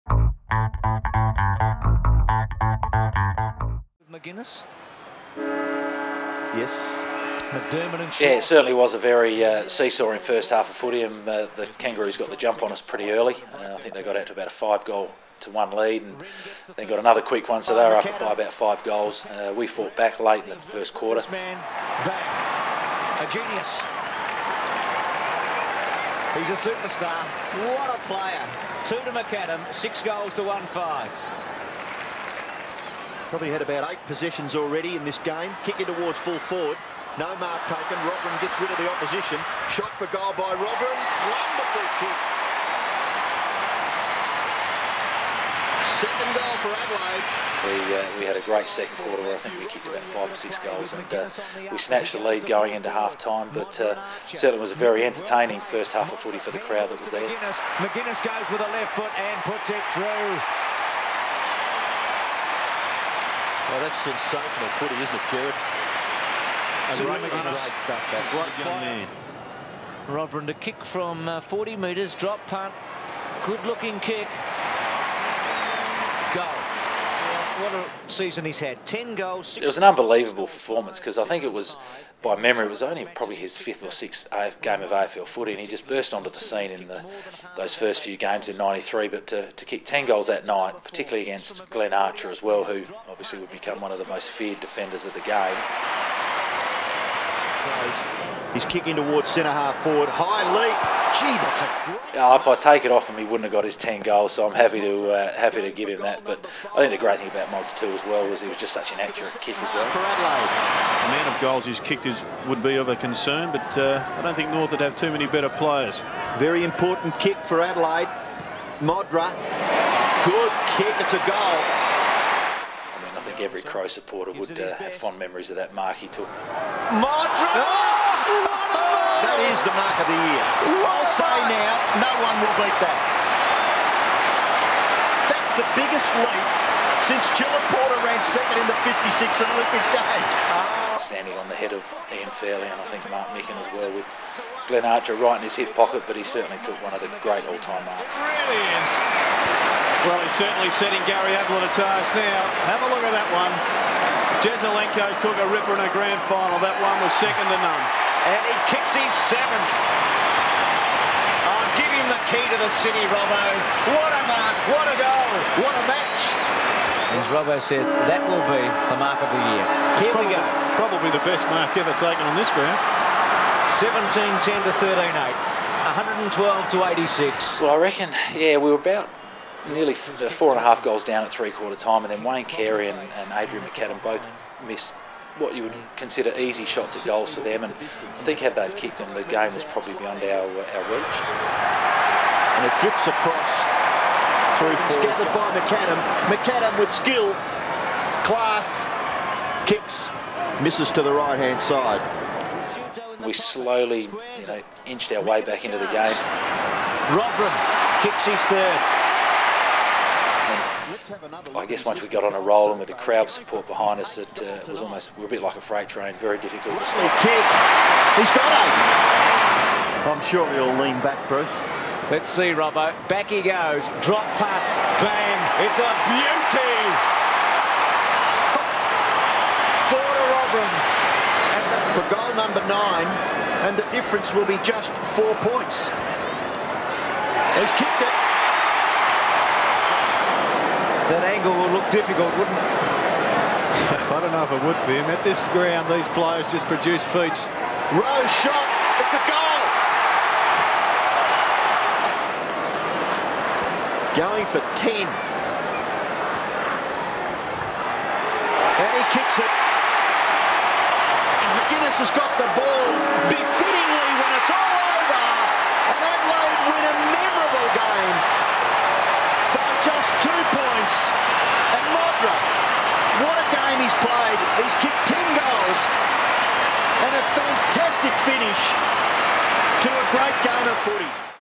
Flashback: Kangaroos - Modra's mark and 10 goals
In 1993, the North Melbourne Kangaroos traveled to Football Park to take on the Crows. Matthew Robran reflects on a great game.